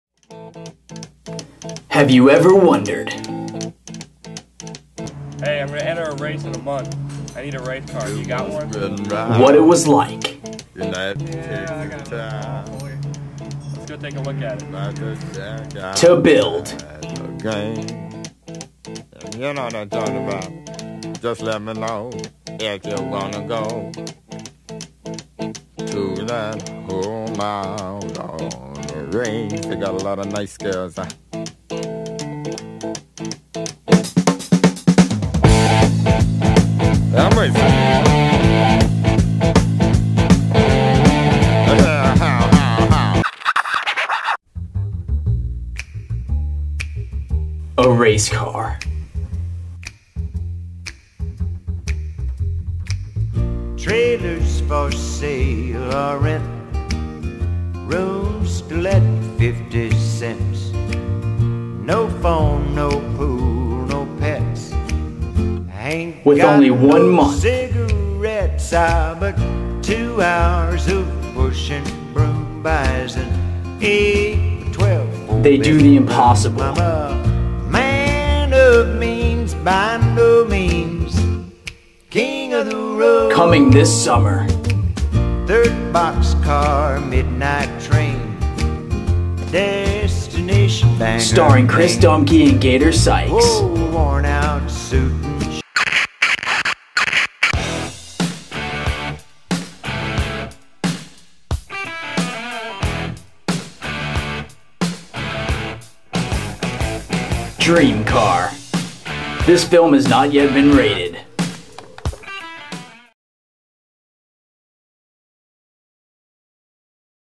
Dream Car- Movie trailer BMW
a very funny movie trailer which involves transforming a Buick to a racing BMW made in the Durant High School TV production dept.